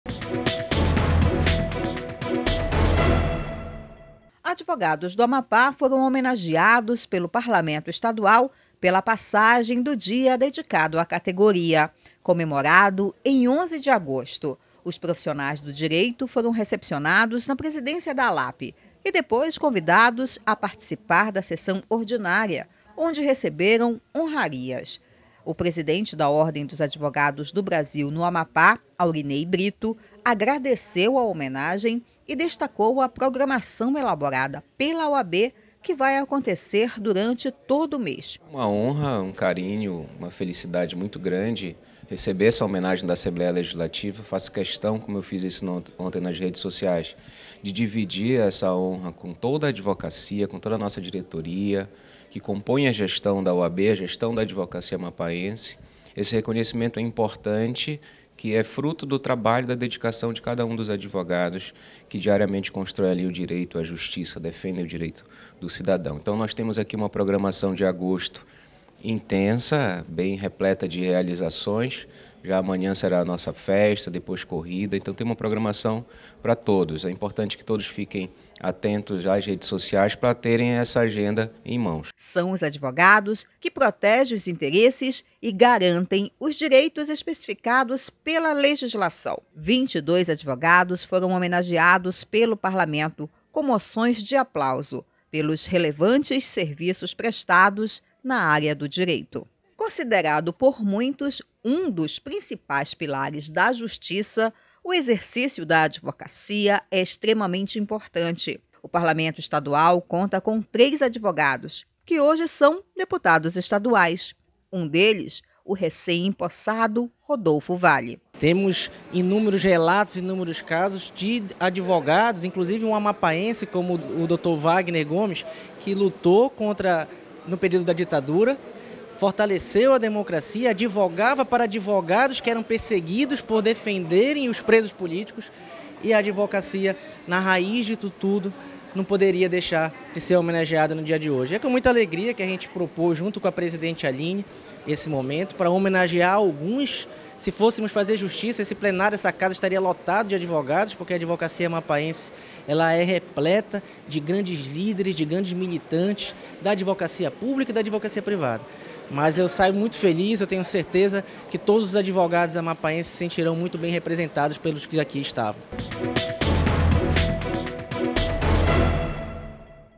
Advogados do Amapá foram homenageados no parlamento estadual pelo dia 11 de agosto, dia do advogado.
Os profissionais do direito foram recepcionados  na presidência da Alap  e depois convidados a participar da sessão ordinária onde receberam honrarias
 O parlamento estadual conta com 03 advogados que hoje são deputados estaduais